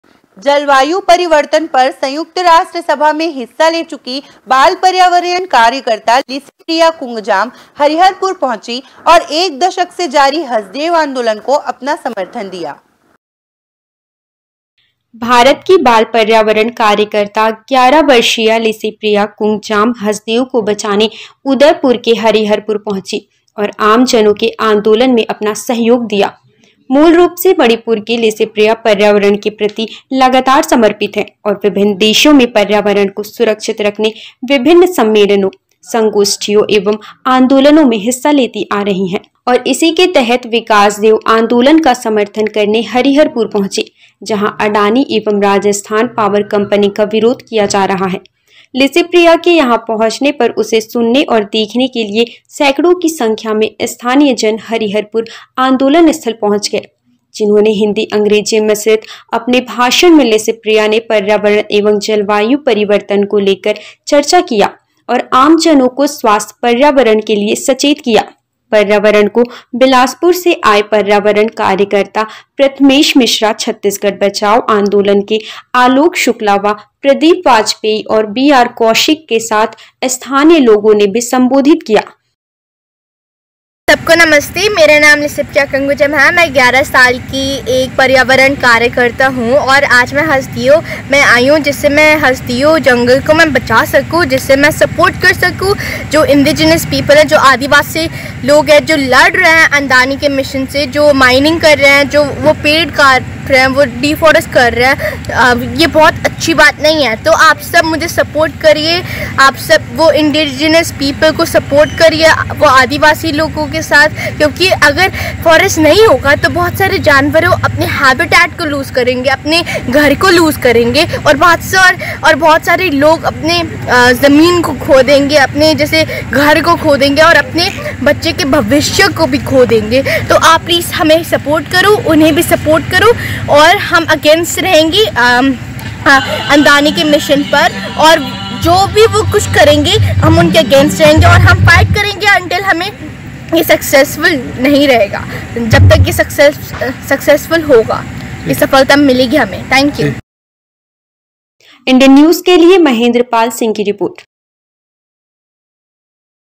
हसदेव आंदोलन का समर्थन करने सरगुजा पहुंची "लिसिप्रिया कंगुजम".......@IndianNewsMPCG
Indian News : जलवायु परिवर्तन पर संयुक्त राष्ट सभा में हिस्सा ले चुकी बाल पर्यावरण कार्यकर्ता लिसिप्रिया कुंगजाम  हरिहरपुर पहुंची और एक दशक से जारी हसदेव  आंदोलन को अपना समर्थन दिया ।

 भारत की बाल पर्यावरण कार्यकर्ता 11 वर्षीया लिसिप्रिया कुंगजाम हसदेव को बचाने उदयपुर के हरिहरपुर पहुंची और आमजनों के आंदोलन को अपना सहयोग दिया। मूल रूप से मणिपुर की लिसिप्रिया पर्यावरण के प्रति लगातार समर्पित हैं और विभिन्न देशों में पर्यावरण को सुरक्षित रखने विभिन्न सम्मेलनों, संगोष्ठियों एवं आंदोलनों में हिस्सा लेती आ रही है। और इसी के तहत विकास देव आंदोलन का समर्थन करने हरिहरपुर पहुंची जहां अडानी एवं राजस्थान पावर कम्पनी का विरोध किया जा रहा है। लिसिप्रिया के यहां पहुंचने पर उसे सुनने और देखने के लिए सैकड़ों की संख्या में स्थानीय जन हरिहरपुर आंदोलन स्थल पर मौजूद रहे। जिन्होंने हिंदी-अंग्रेजी मिश्रित अपने भाषण में लिसिप्रिया ने पर्यावरण एवं जलवायु परिवर्तन को लेकर चर्चा किया और आमजनों को स्वस्थ्य पर्यावरण के लिए सचेत किया।कार्यक्रम को बिलासपुर से आए पर्यावरण कार्यकर्ता प्रथमेश मिश्रा छत्तीसगढ़ बचाओ आंदोलन के आलोक शुक्ला  व प्रदीप वाजपेई,और बीआर कौशिक, के साथ स्थानीय लोगों ने भी संबोधित किया। महेंद्र पाल सिंह की रिपोर्ट